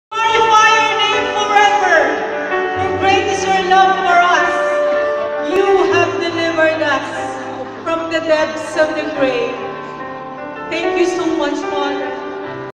National Marian Congress 2017 (Day 2) Part 8 of 9
DAY 2: July 8, 2017 Saturday 8:00 AM - 5:00 PM. "The 2-DAY NATIONAL MARIAN CONGRESS on the Centennial of the Apparitions of Fatima" in SMX Convention Center, Mall of Asia, Pasay City. Runtime: 0:12.